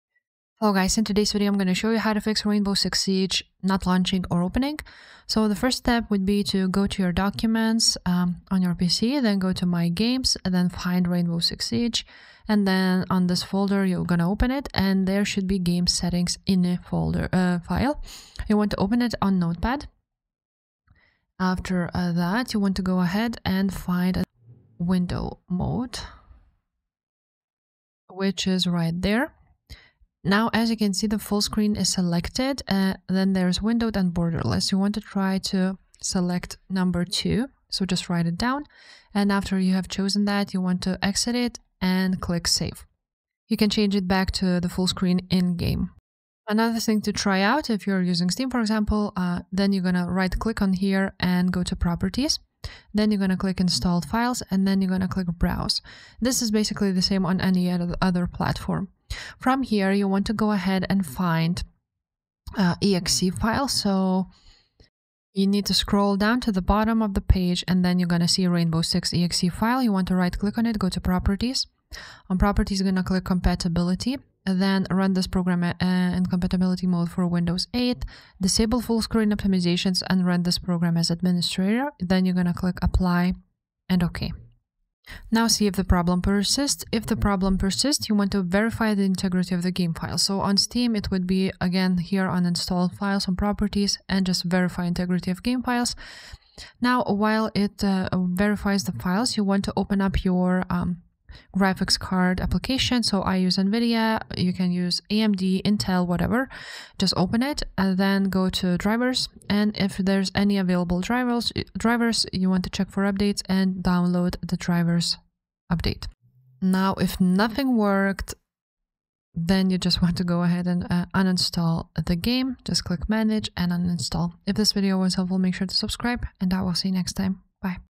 How To Fix Rainbow Six Siege Not Launching Or Opening
In this video I show you How To Fix Rainbow Six Siege Not Launching Or Opening!